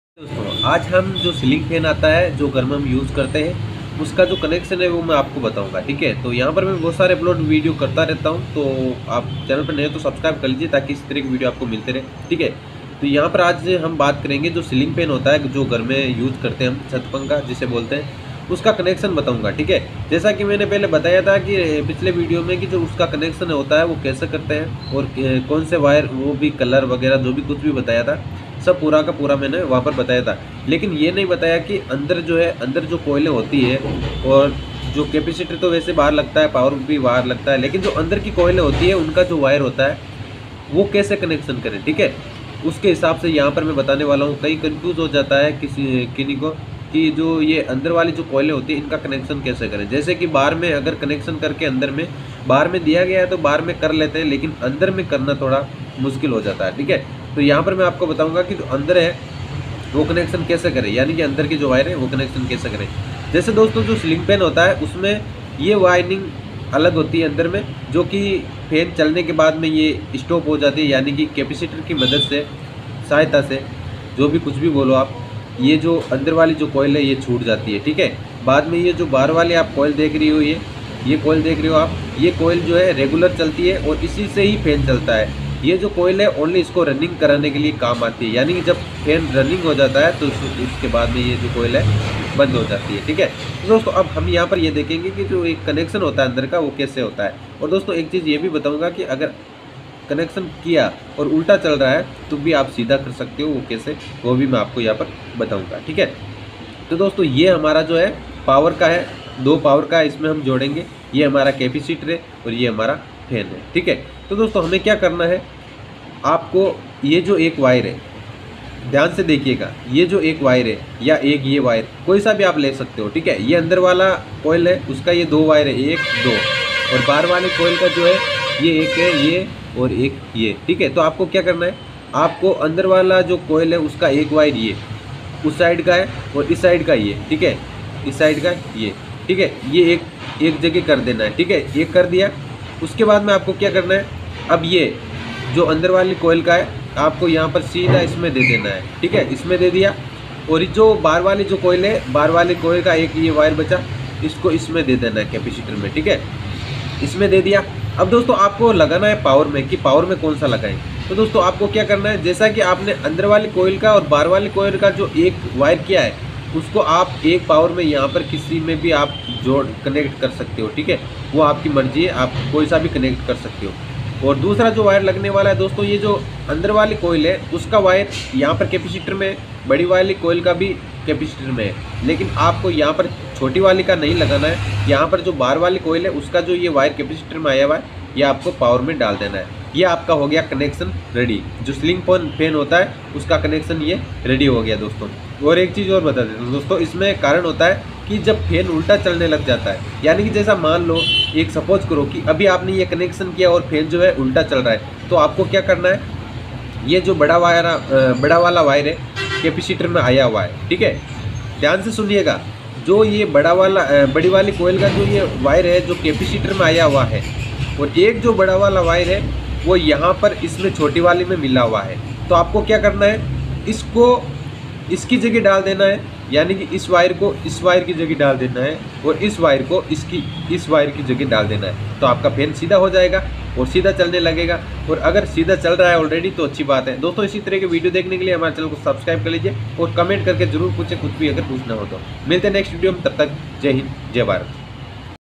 fan connection under winding उल्टा सीधा चलता है तो
fan connection kaise karen

is video me fan under winding and connection betaya gya h